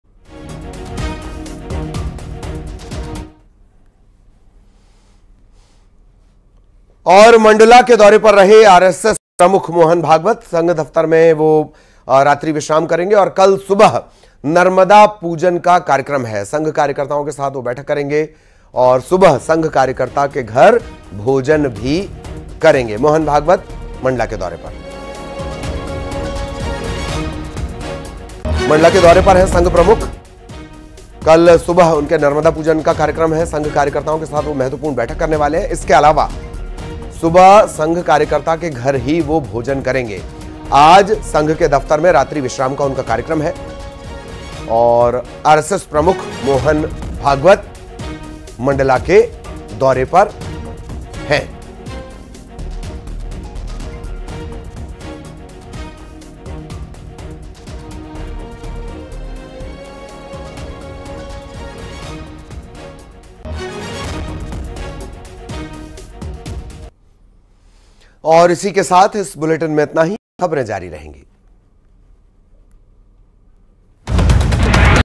Mohan Bhagwat Mandla Visit : मोहन भागवत करेंगे नर्मदा पूजन | Breaking News | RSS | Narmada River
मोहन भागवत करेंगे नर्मदा पूजन, साथ ही कल लेंगे मीटिंग